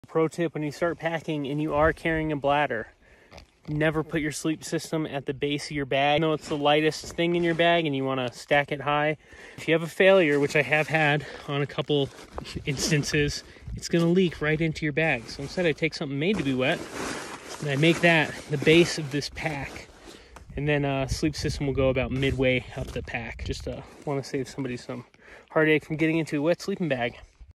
NEVER Sleep in a Soaked Bag Again!
Packing for a hunt or camping trip? Avoid rolling out a wet sleeping bag by not packing your sleeping system in the bottom of your pack - if your water bladder fails, it will leak right into your sleeping bag. Put something made to be wet in the bottom instead!